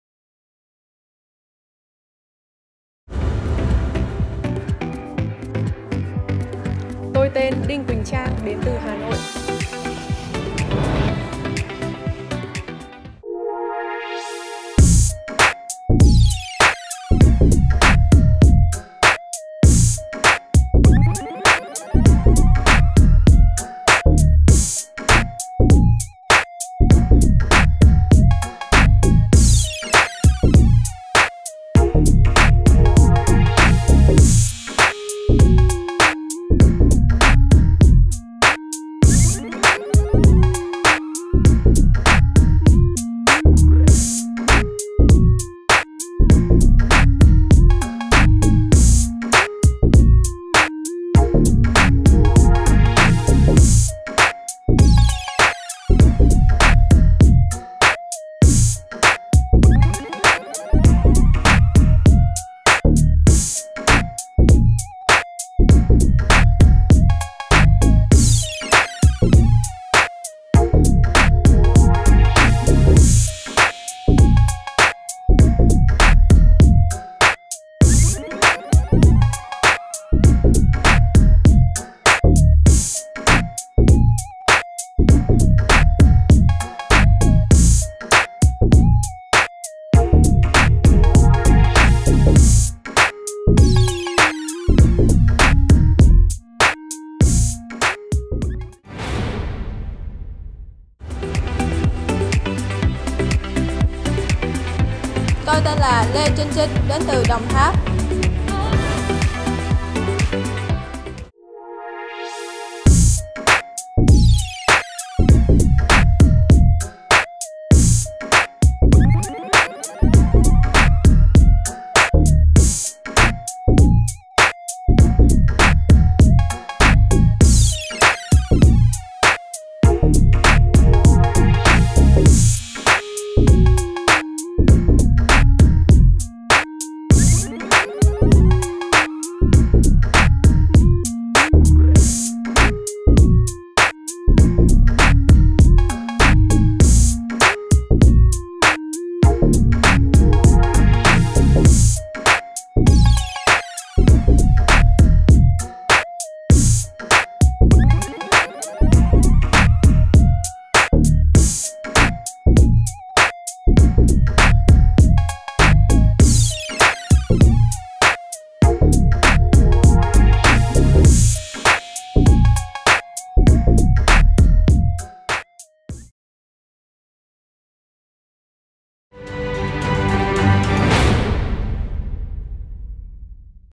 Đinh Huỳnh Trang SBD 025 VS Lê Trinh Trinh SBD 028 - Phần thi Bikini MWVN 2019 - Bạn sẽ chọn ai?
Vậy là đã xác định được danh sách 39 cô gái xinh đẹp, tài năng lọt vào vòng chung kết cuộc thi Miss World Việt Nam 2019. Hãy cùng ngắm nhìn lại hành trình xuất sắc ở đêm chung khảo 2 miền của các thí sinh.